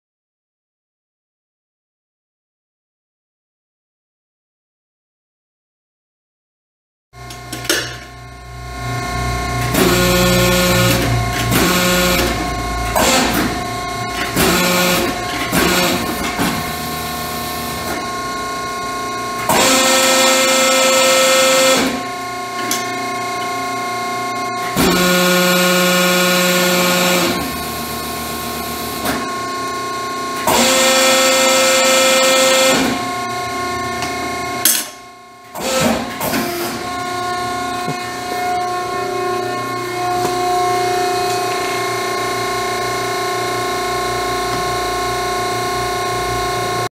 Betenbeder 12957 press brake for sale | no-reserve Internet auction February 7, 2018
View a detailed listing of this item and place bids

Betenbeder 12957 press brake
97 hours on meter
Baldor LN25157 electric motor
20 HP
220V
Foot control
146" rake
Work light
Serial 81360
Buyer is responsible for all aspects of removal.
It's located in Fowler, CO, and we're selling it to the highest bidder regardless of price.